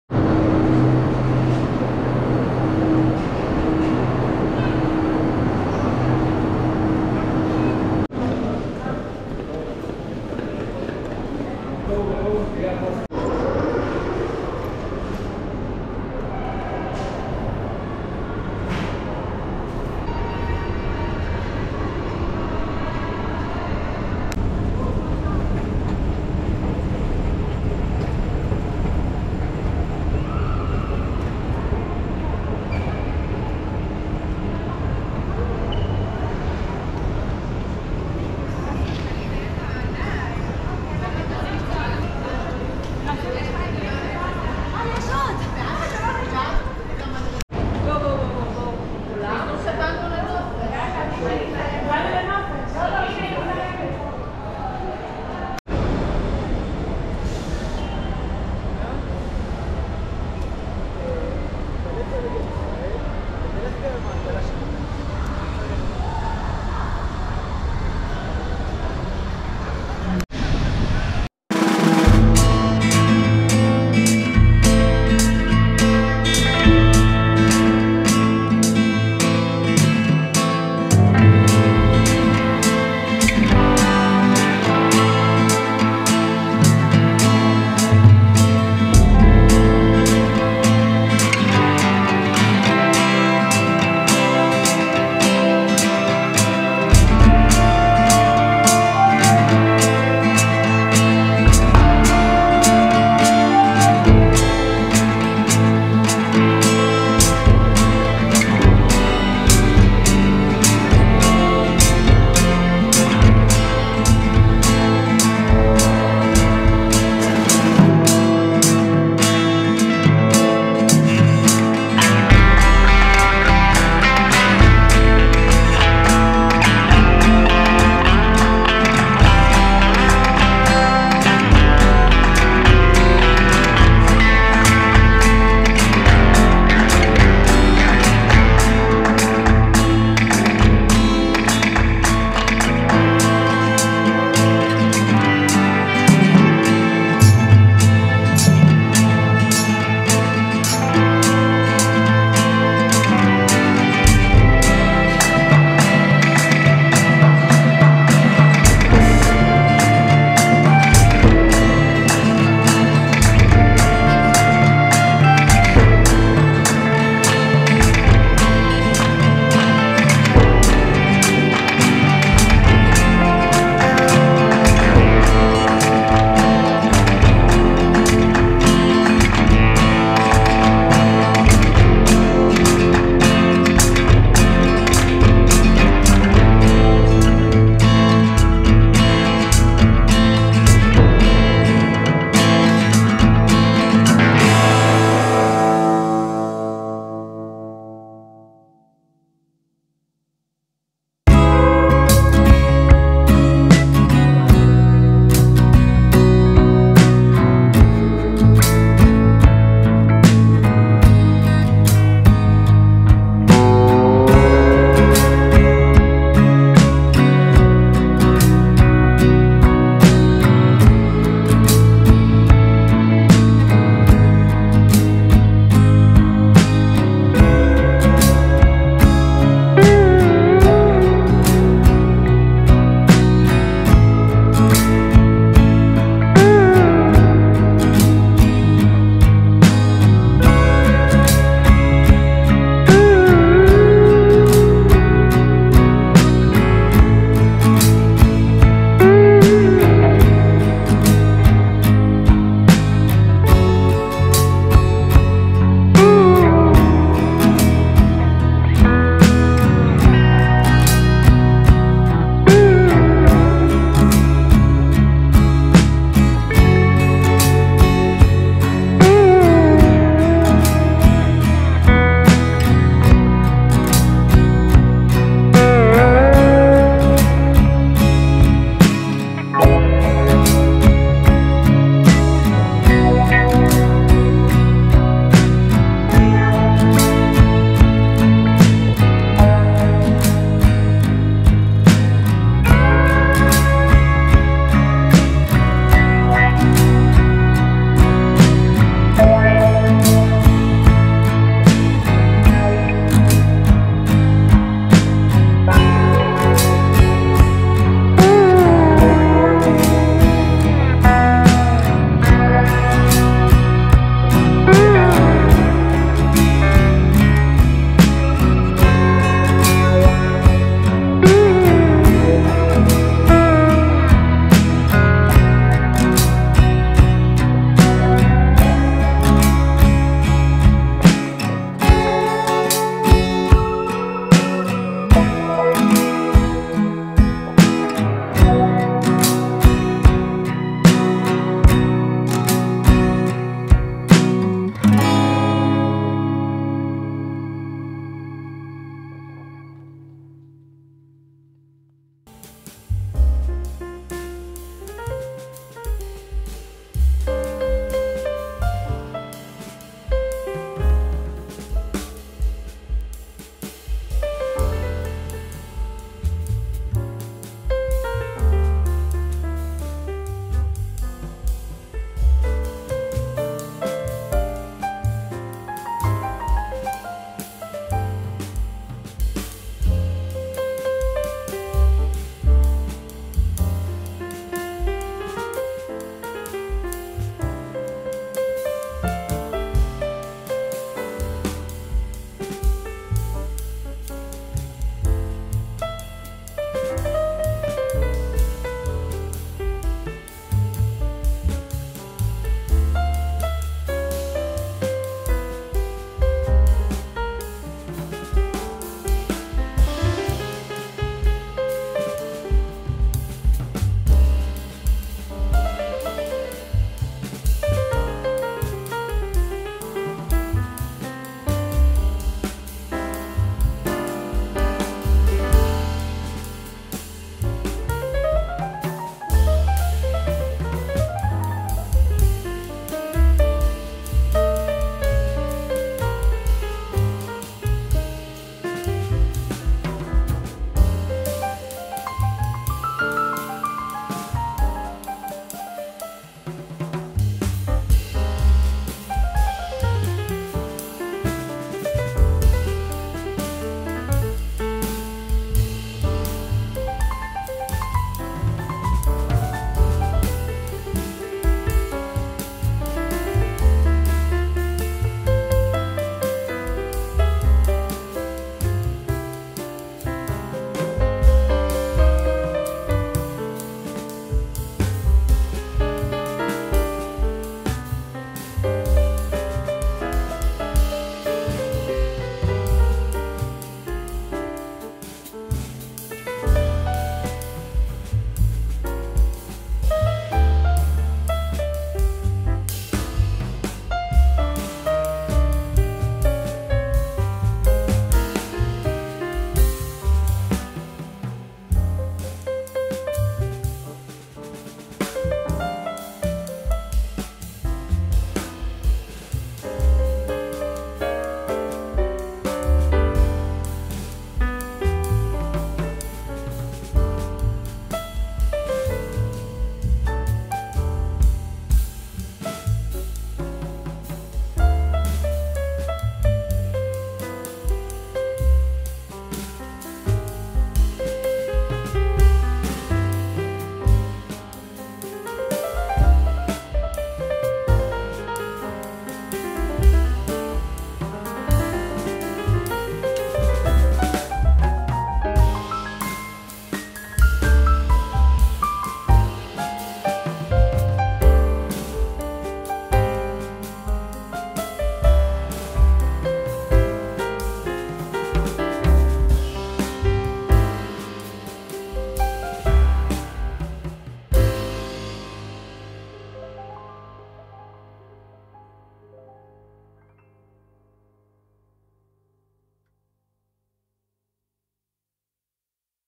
Jerusalem. Yitzhak Navon train station. Walking along Jaffa Street. Иерусалим прогулка по улице Яффо
Join me for a sunny and pleasant morning walk through the busy and colorful streets of the City Center of Jerusalem.
We will cover the entire length of Jaffa Street, from the Yitzhak Navon train station to the entrance to the Old City

Я приглашаю вас присоединиться ко мне на солнечную утреннюю прогулку по оживленным и красочным улицам центра Иерусалима.
Мы пройдем всю улицу Яффо, от вокзала Ицхак Навон до входа в старый город.